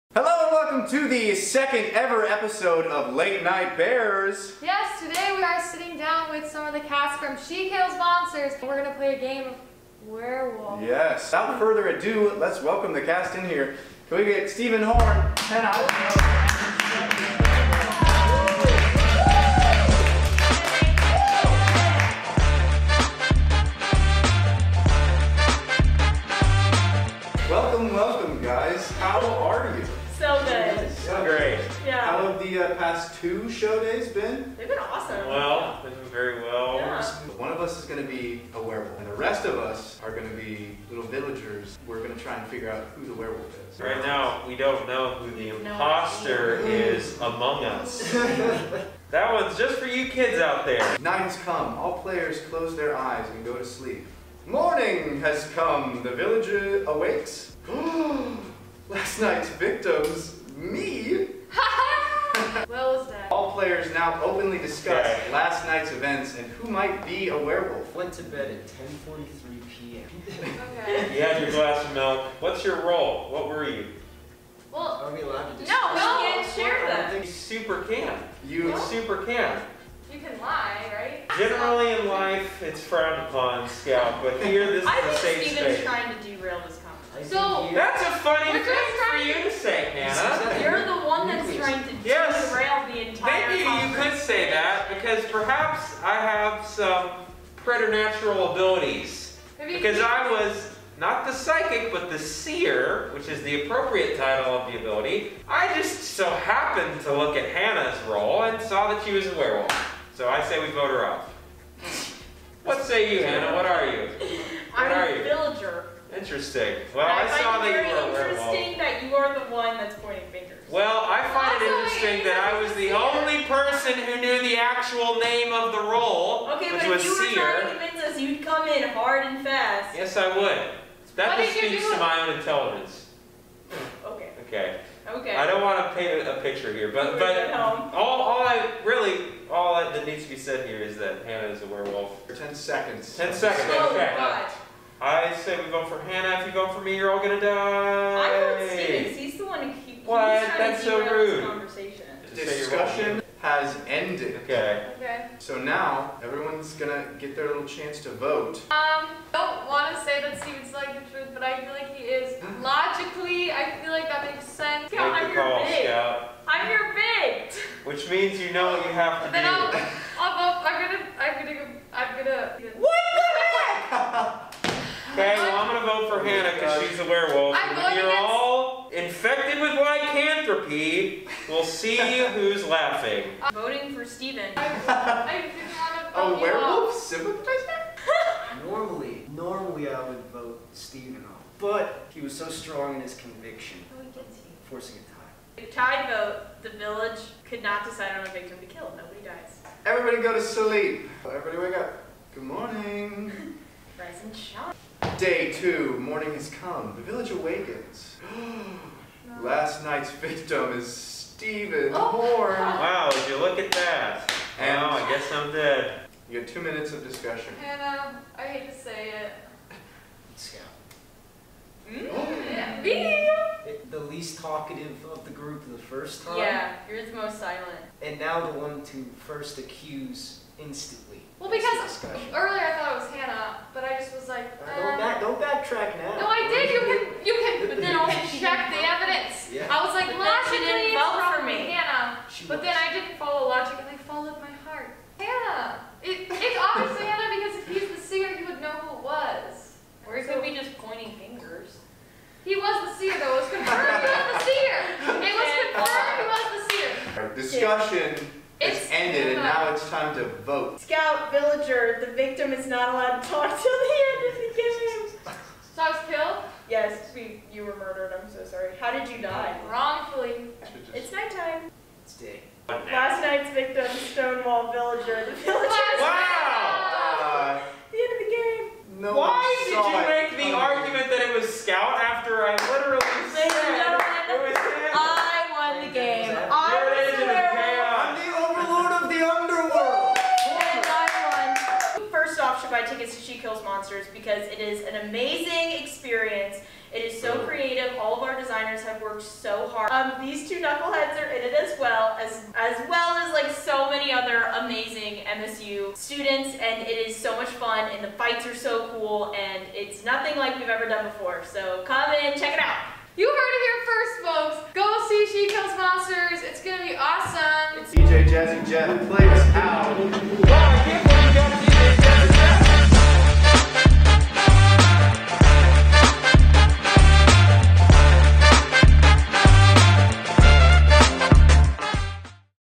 T&D Wrap & She Kills Monsters!
In this week's episode Will & Scout play a game with the cast of She Kills Monsters. Find out who is innocent and who is the werewolf!